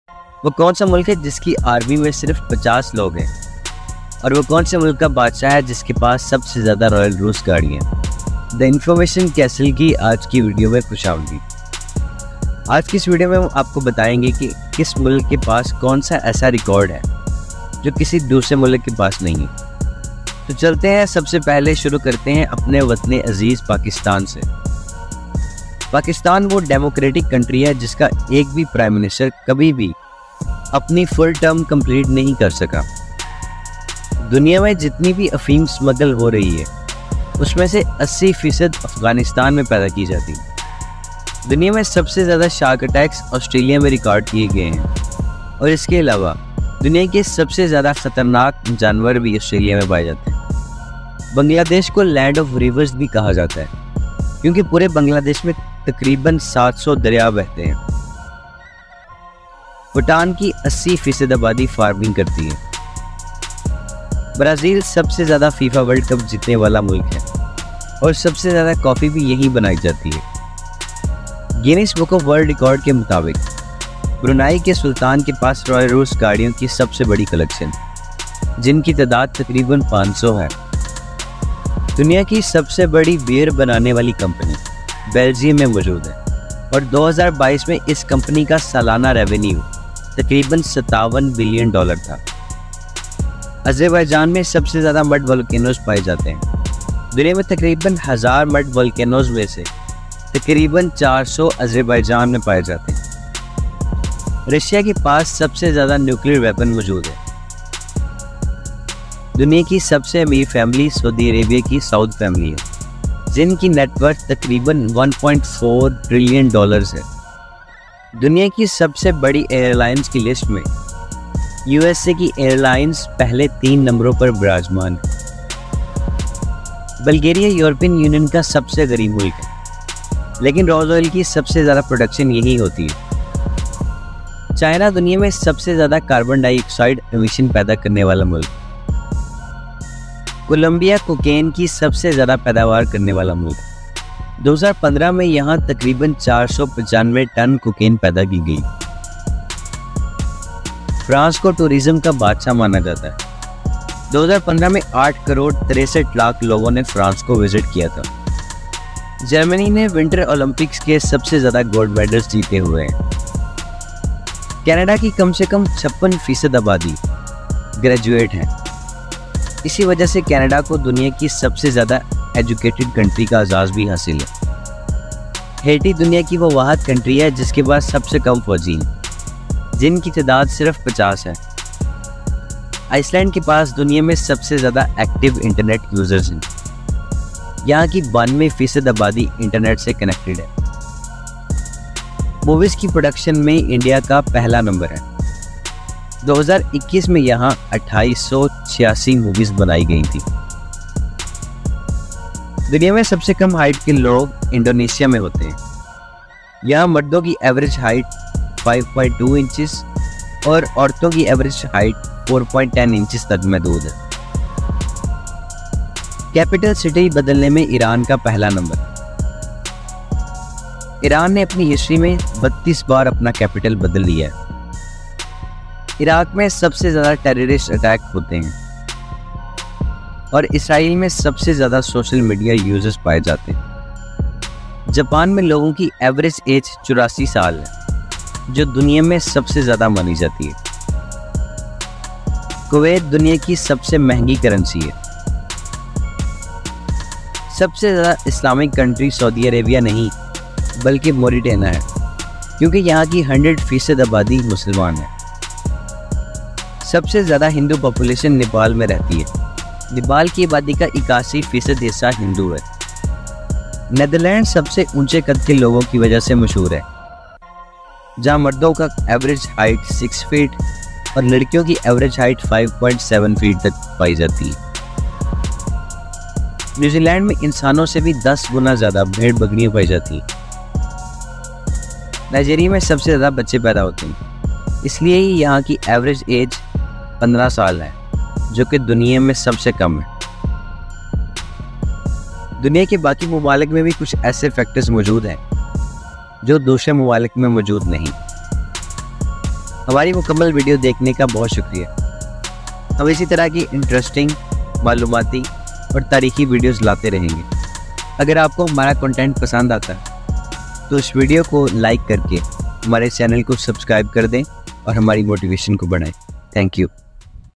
Different World Records of Different Countries
Discover a world beyond imagination as we delve into the most astonishing records set by individuals and teams from various nations. From the tallest structures to the fastest feats, join us on a journey through unparalleled achievements that have left their mark on history. Explore this compilation of extraordinary world records, showcasing the diverse talents and determination of people from different corners of the earth. Prepare to be amazed as we unveil the unparalleled accomplishments that have earned their place in the global record books.
Join the conversation and let us know your favorite record from the video in the comments below! Together, let's celebrate the remarkable accomplishments that make our world truly unique.